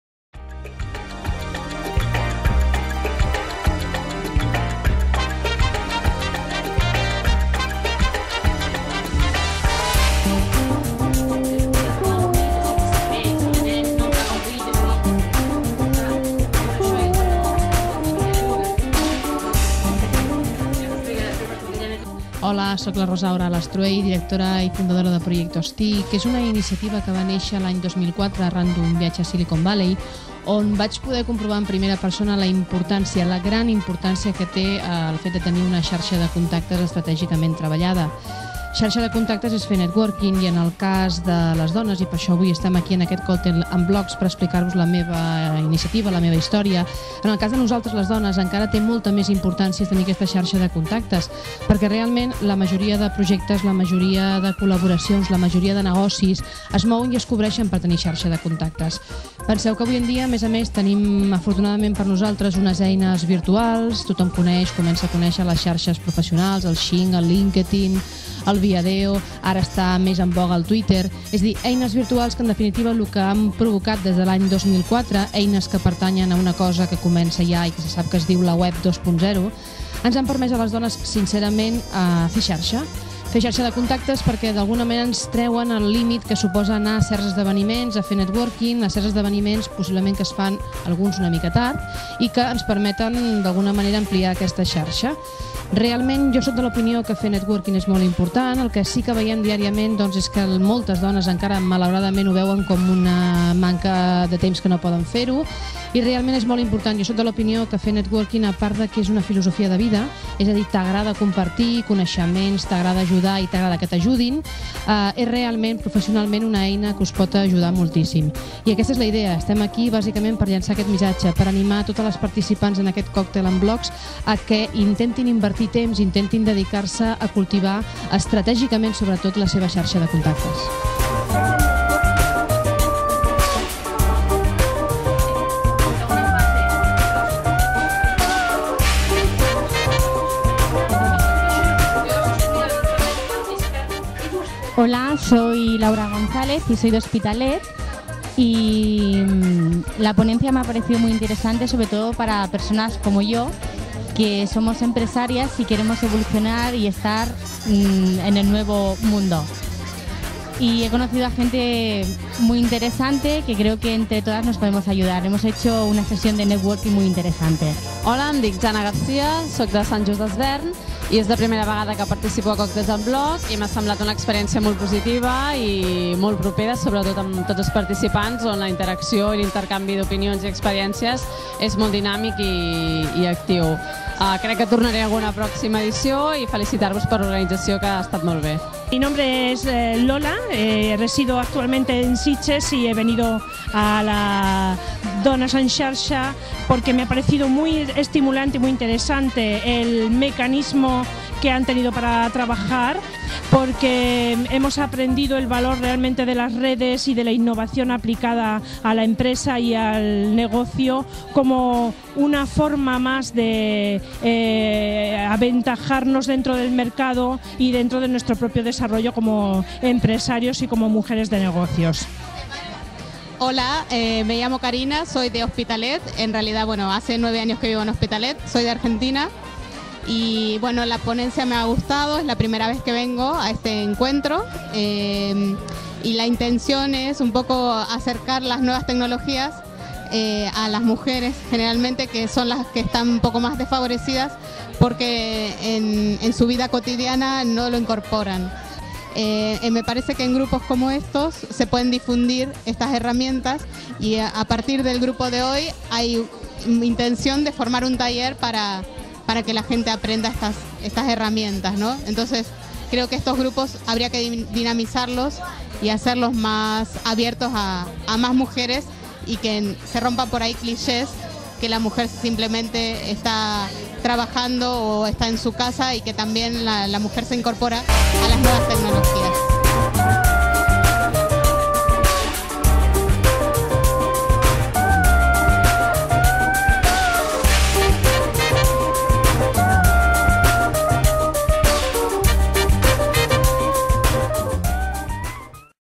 IV Còctels & Blocs 14/04/2011 Barcelona
Una trentena de dones ens van acompanyar en el IV Còctels & Blocs de Dones en Xarxa. L'experta en networking, Rosaura Alastruey, fou la convidada d'excepció.

Autora: Patrícia Fernàndez